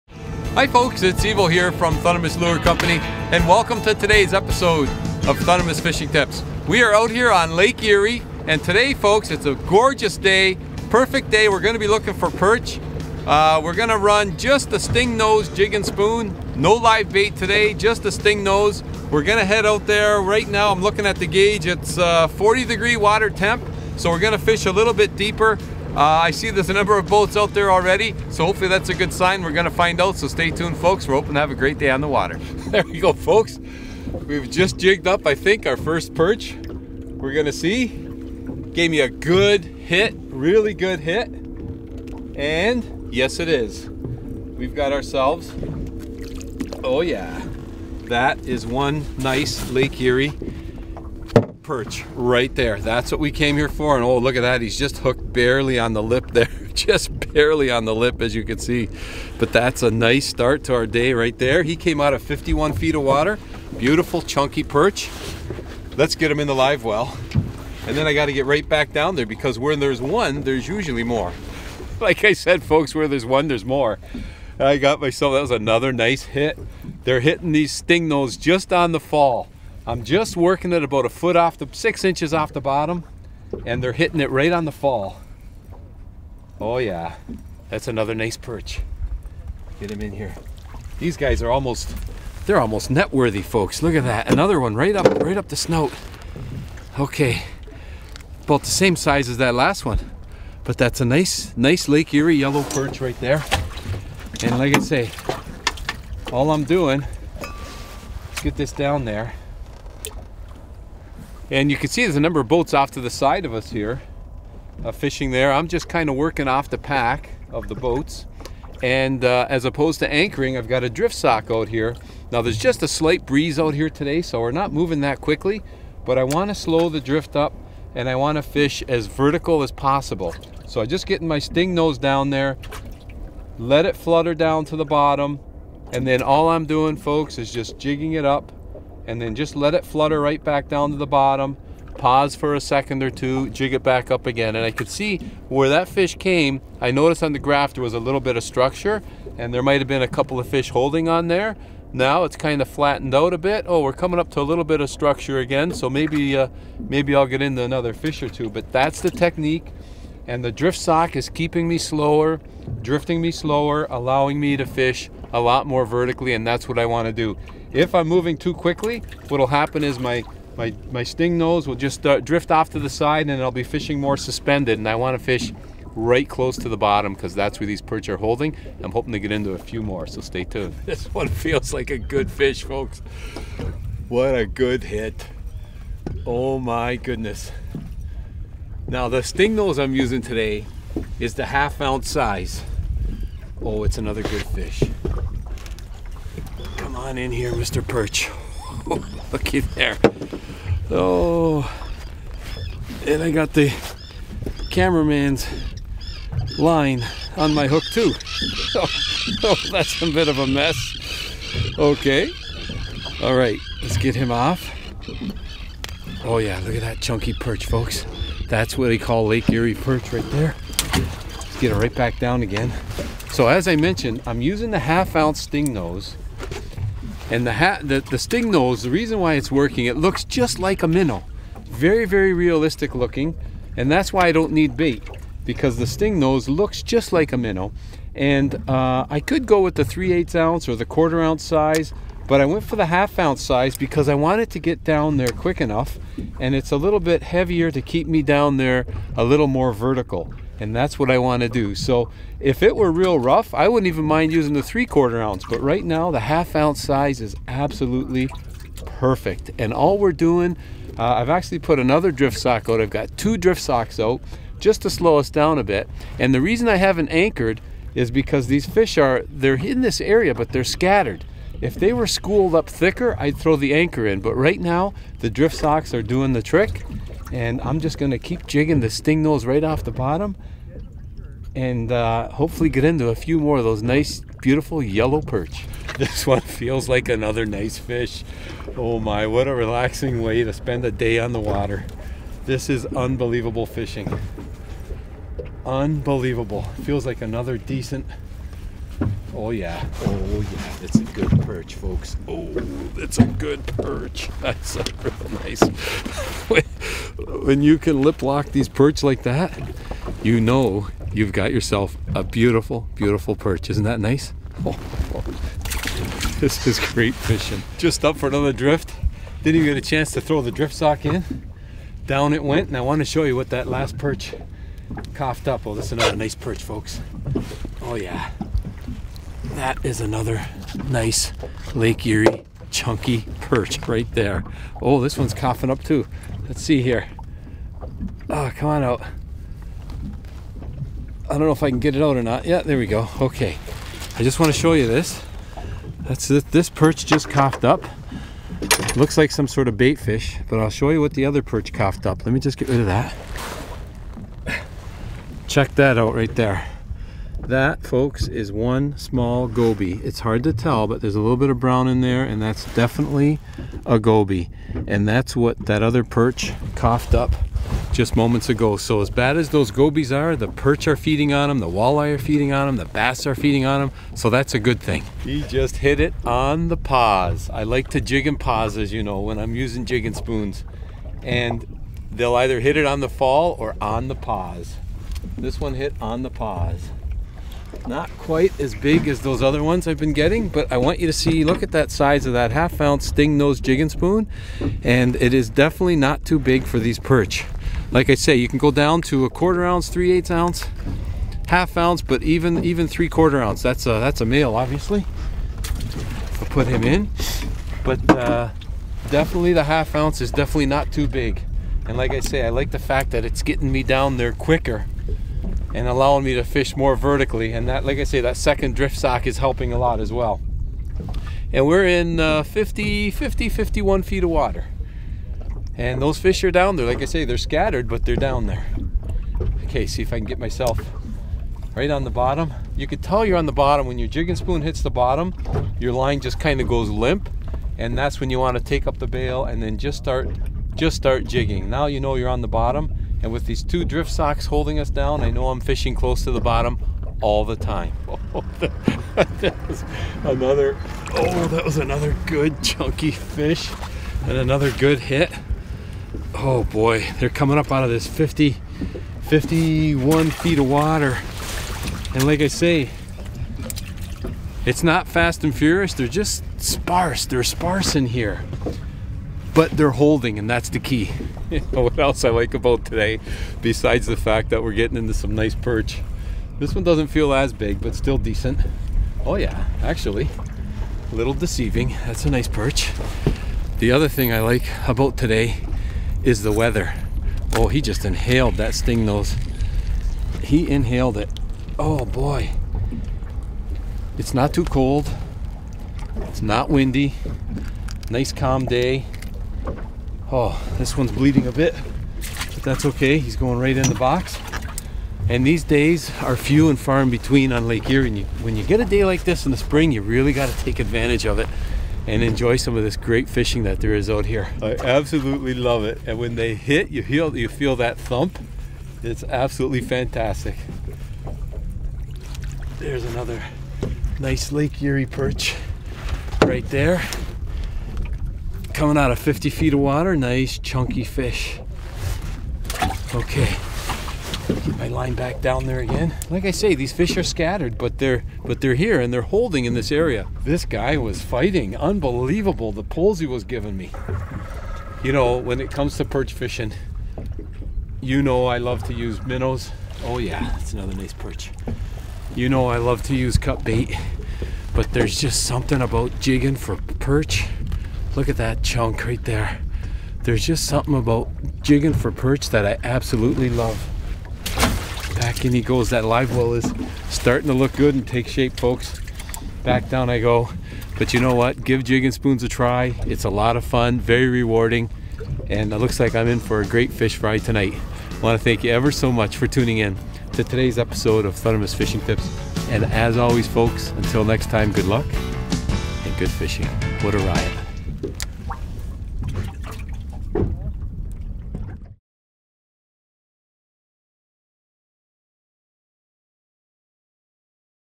Jumbo Perch Overload! Using the Simple Stingnose Jigging Spoons
We're out here on Lake Erie fishing about 50 feet deep and hammering those jumbo perch! We're using the Stingnose jigging spoons today.